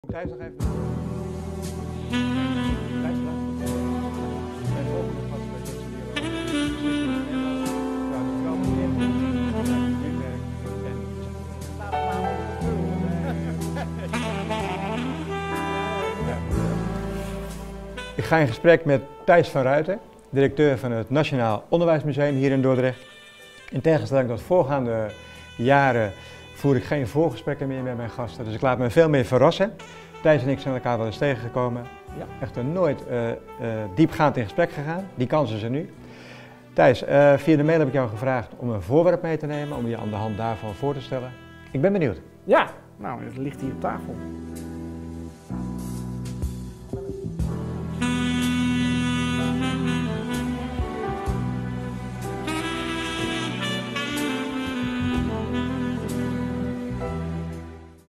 Dordtse Dialogen zondag 10 september bij TV Dordrecht
Dordtse Dialogen zondag 10 september bij TV Dordrecht met als gast Tijs van Ruiten, directeur van het Nationaal Onderwijsmuseum.